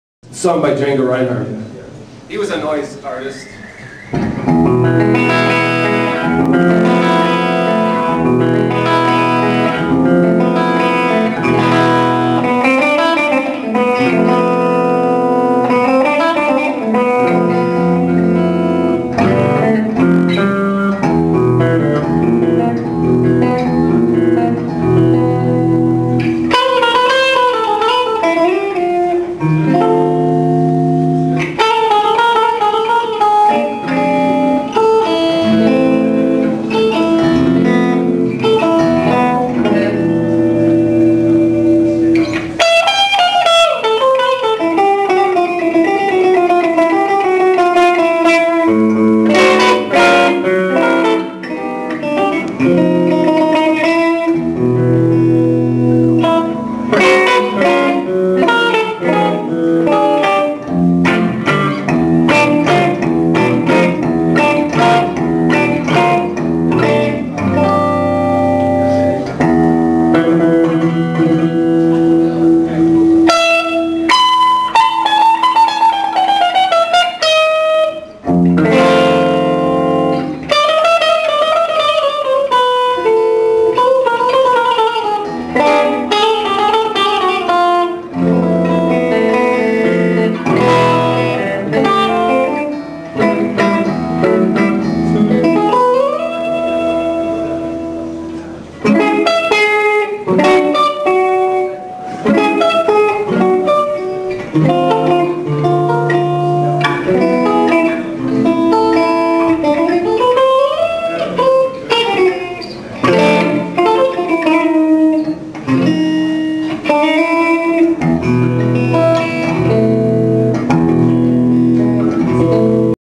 Sir Richard Bishop @Hafenklang Hamburg (DE) 03/04/08
A short clip of Sir Richard Bishop playing at 'Hafenklang' in Hamburg, Germany. He opened for EARTH and did a great job.

This is a rather quiet tune, in fact he's a real shredder.